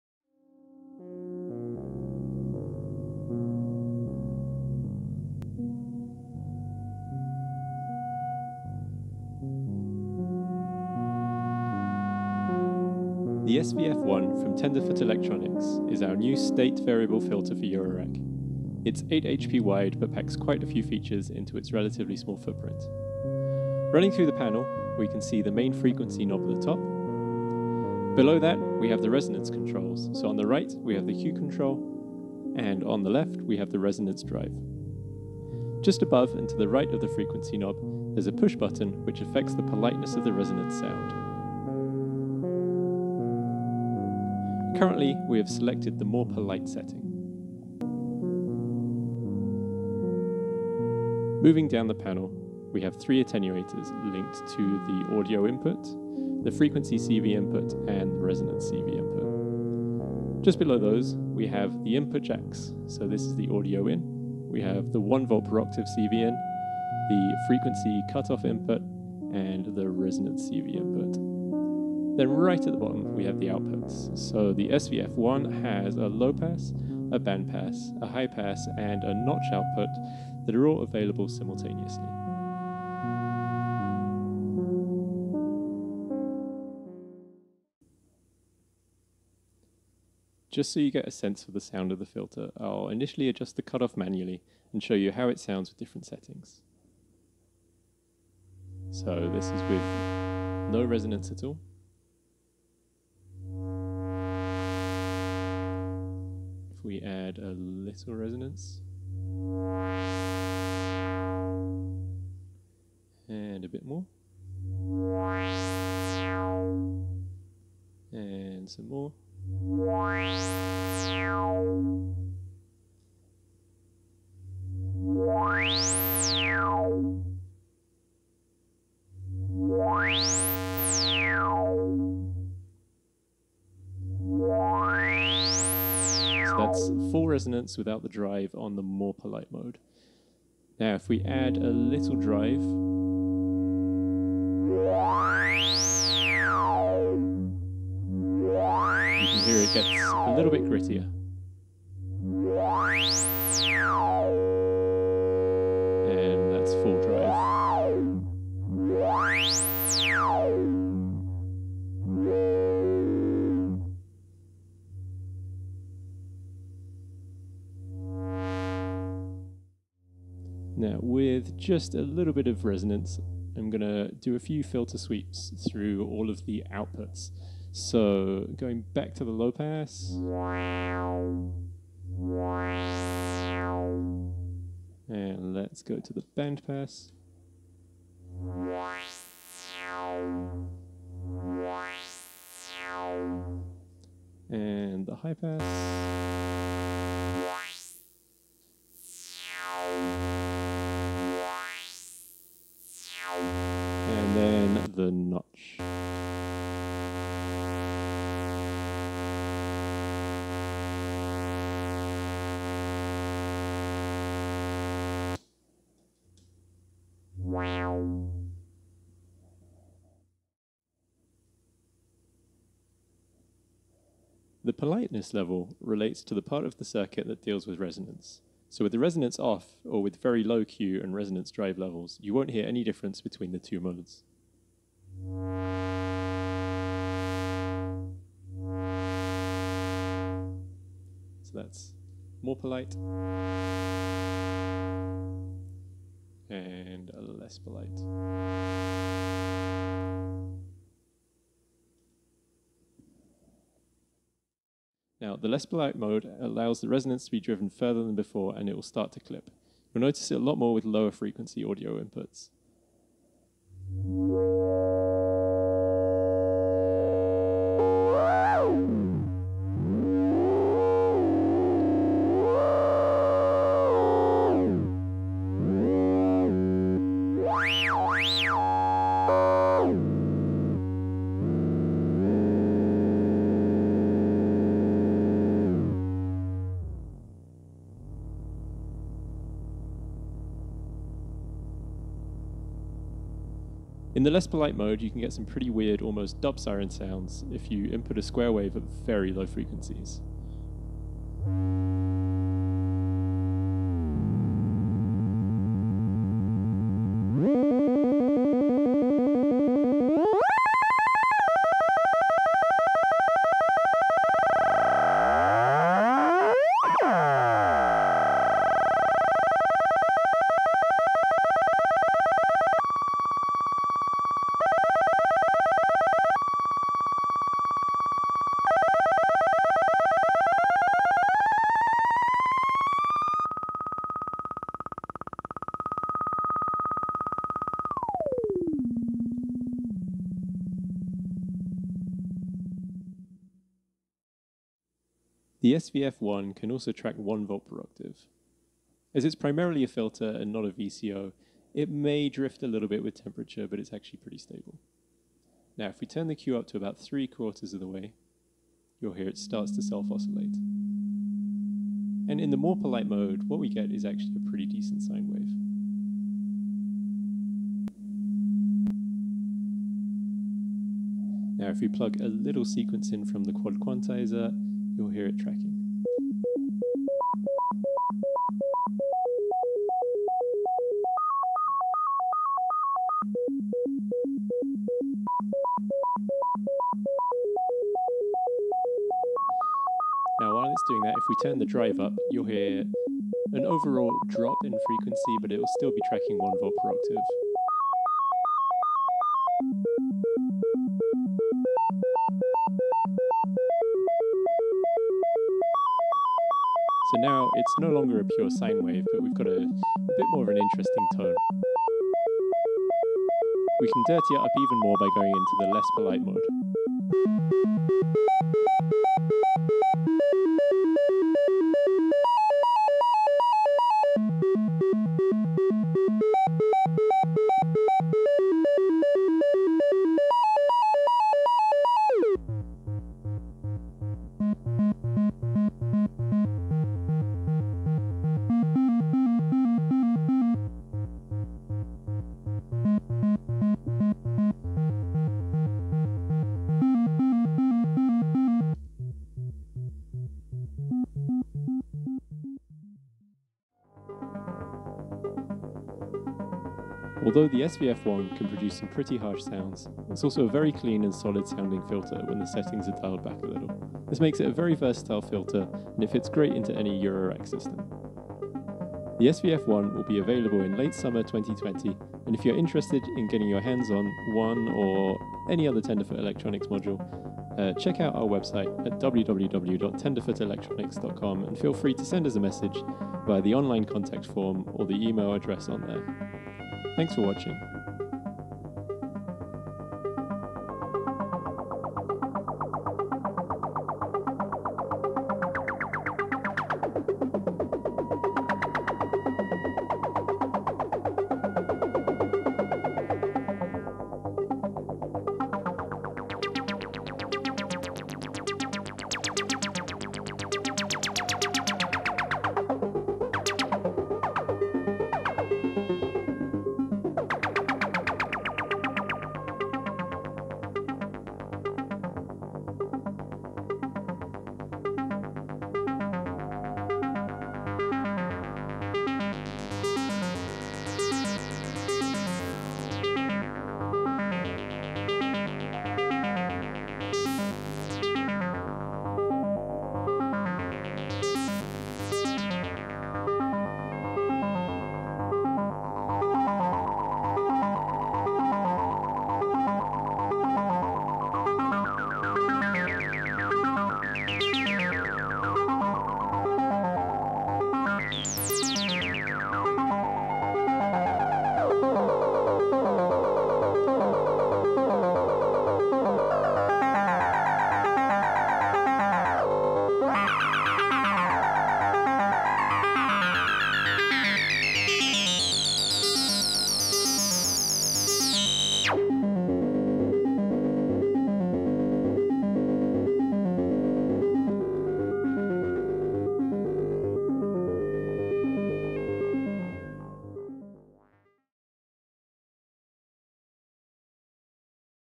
SVF-1 Filter Overview
A quick run-through of the features of the SVF-1, the new state-variable filter from Tenderfoot Electronics.

8HP

Available late summer 2020.